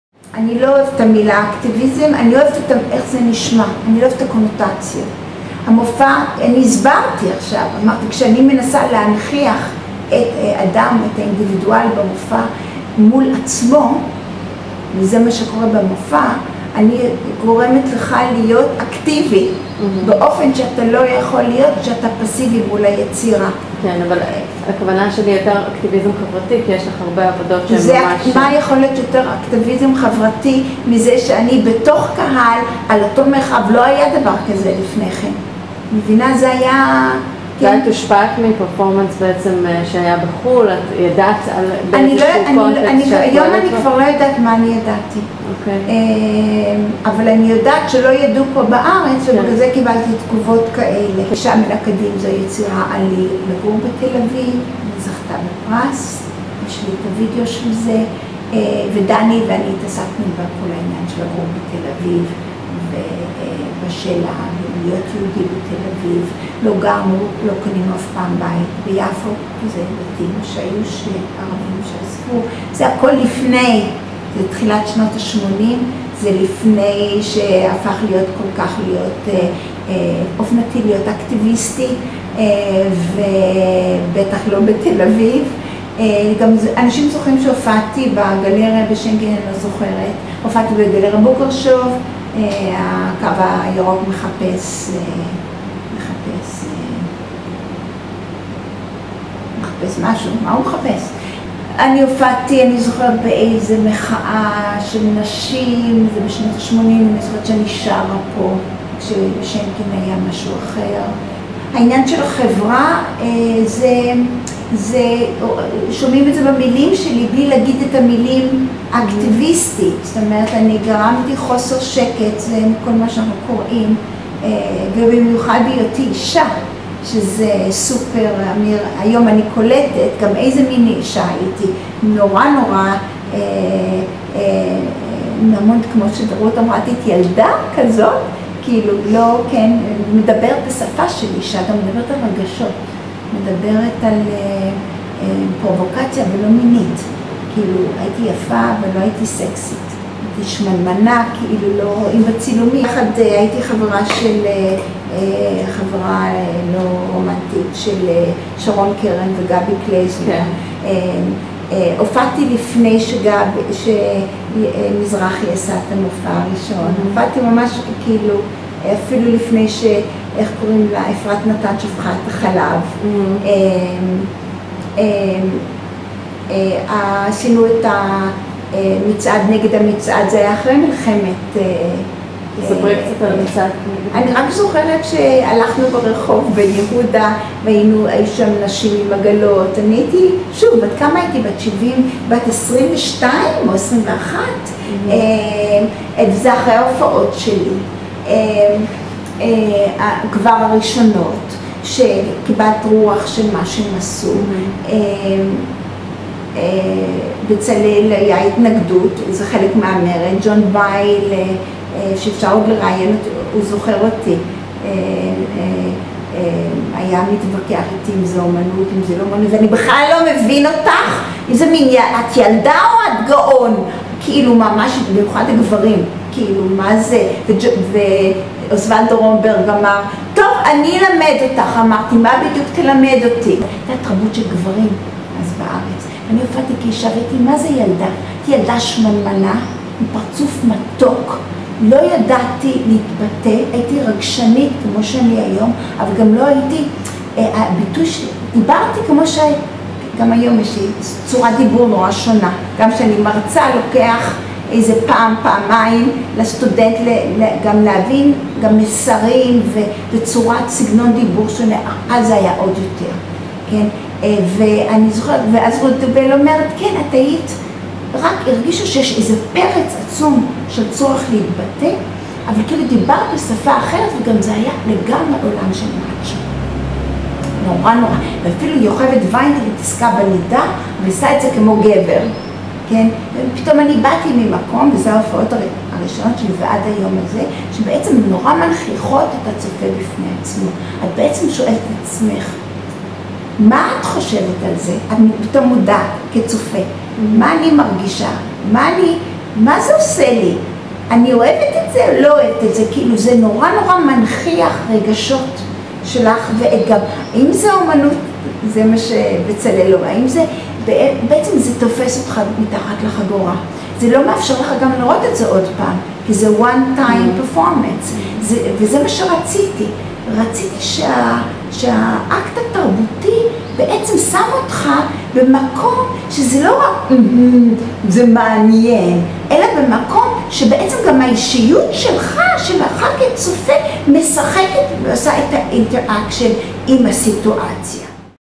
Adina Bar-On_Interview
ראיון עם עדינה בר און, מחלוצות אמנות המופע בארץ. עדינה מתארת את חווייתה כאמנית-אישה, אשר פעלה במדיום חדש וחתרני לאותה תקופה. היא מתייחסת לאופן שבו המופע עצמו הוא פעולה אקטיביסטית, כזו המנסה לשנות מוסכמות חברתיות, פועלת באמצעות רגש ומייצרת מודעות עצמית, בעיקר לנוכח היותה אישה הפועלת בתרבות פטריארכלית ובשדה הנשלט בעיקר על ידי גברים.

הראיון נערך במסגרת המחקר ההיסטורי על תרבות ואקטיביזם בתל אביב-יפו המלווה את התערוכה "עיר שדה" [אוהבים אמנות 2012].

מראיינת: דינה יקרסון
אוצרת "עיר שדה": לאה אביר